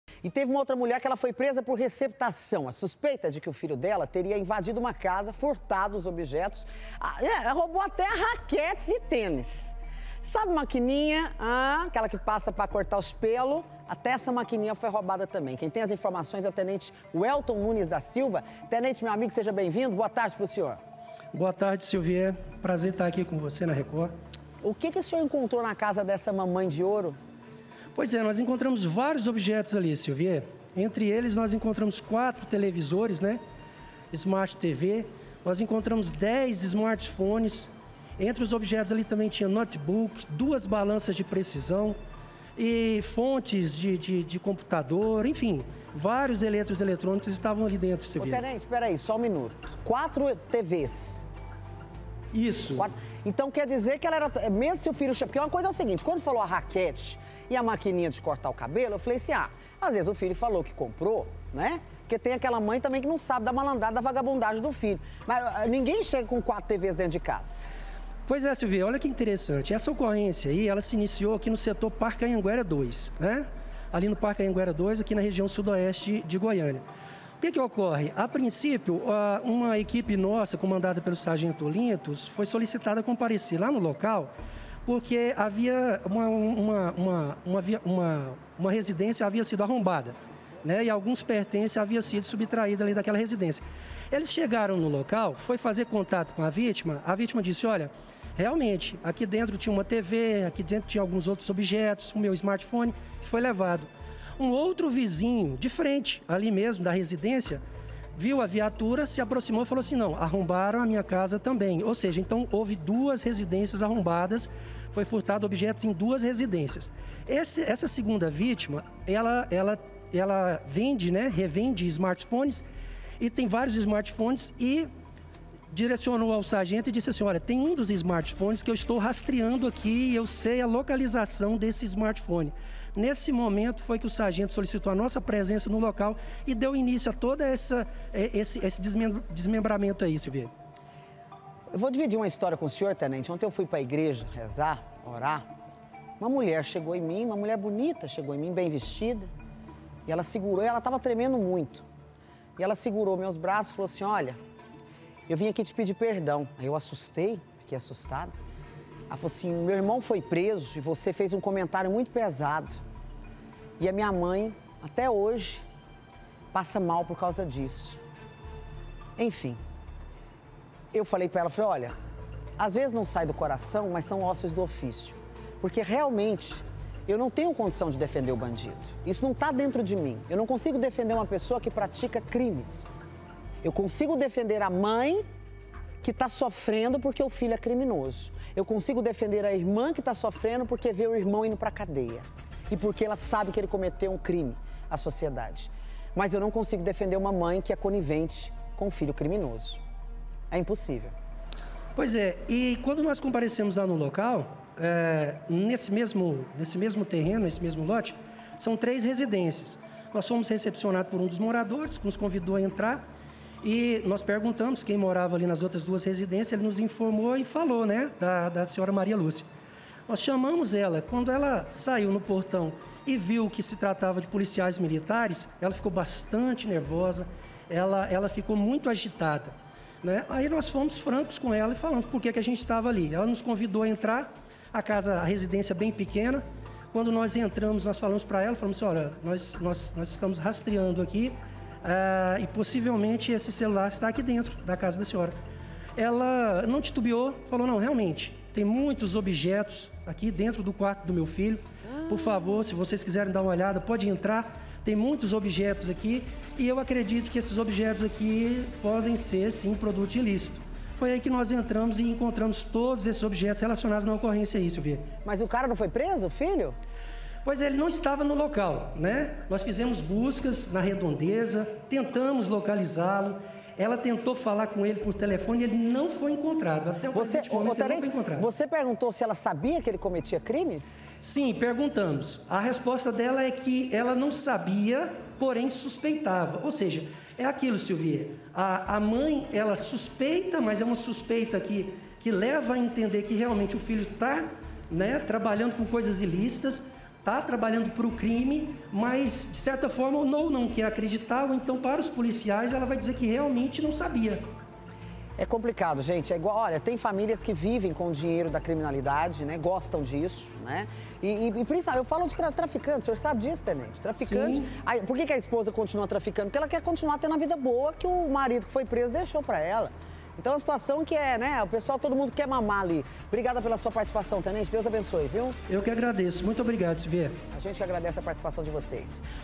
LEVOU ATÉ RAQUETE DE TÊNIS: MÃE É PRESA POR RECEPTAÇÃO; FILHO TERIA INVADIDO CASA
MULHER É PRESA POR RECEPTAÇÃO. A SUSPEITA É DE QUE O FILHO DELA TERIA INVADIDO UMA CASA E FURTADO VÁRIOS OBJETOS, ATÉ A RAQUETE DE TÊNIS E A MAQUININHA DE CORTAR CABELO, MAS AINDA NÃO FOI PRESO. QUEM TEM MAIS INFORMAÇÕES É TENENTE WELTON NUNES DA SILVA.